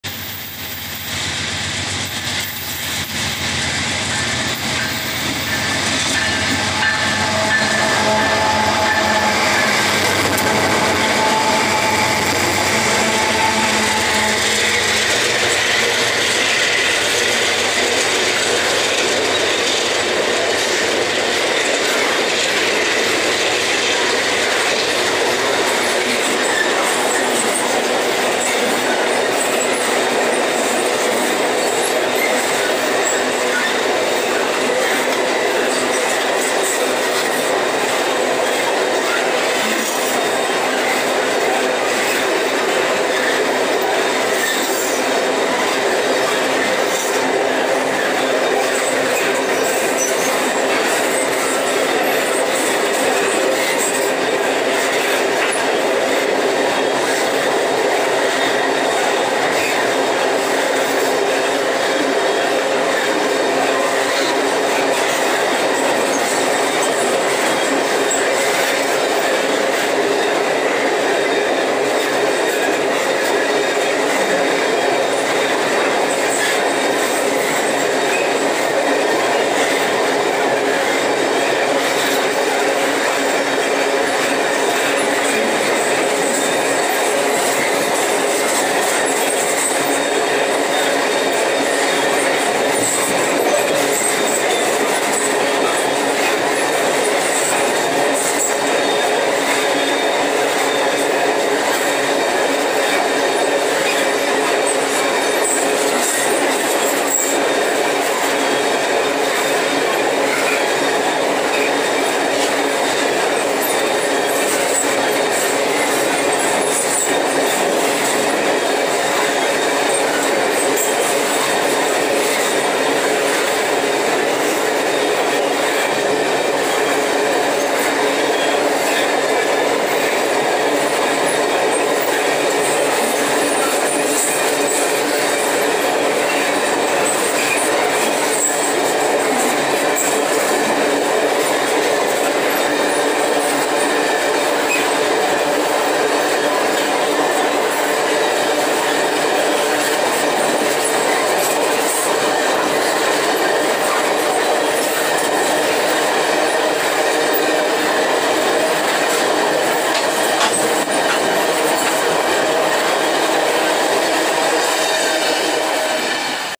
A antepenúltima Locomotiva da numeração 74 a ser fabricada no Brasil para MRS em Três Rios.
MRS - Dupla de AC44I #7404-0 & #7355-8 Minério em Três Rios - RJ.